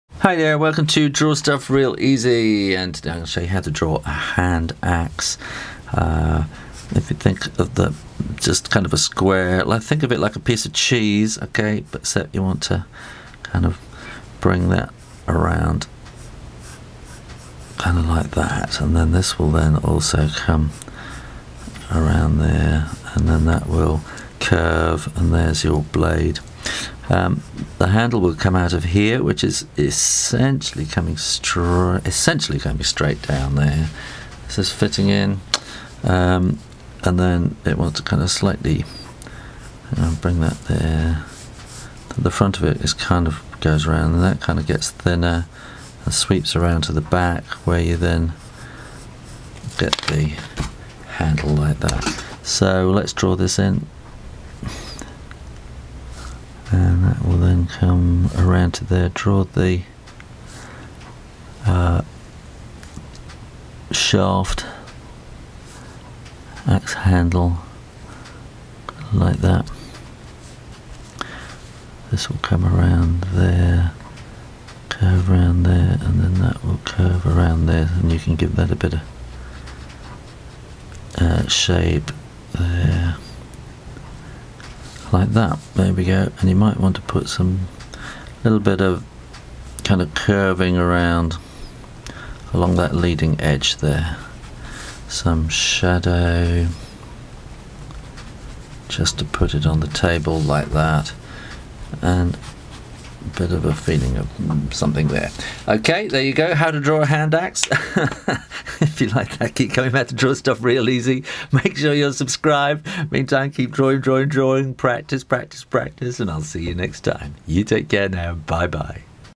How to draw an Axe Real Easy
Get chopping and learn how to draw an Axe Real Easy with award winning illustrator, Shoo Rayner, who has illustrated well over 200 - books for famous authors and for his own stories.

I use a Rotring Tikky Graphic 0.3 pen. The paper is fairly cheap drawing cartridge or bristol paper.

Shoo Rayner has illustrated300 books. See Shoo's books on amazon.com and on amazon.co.uk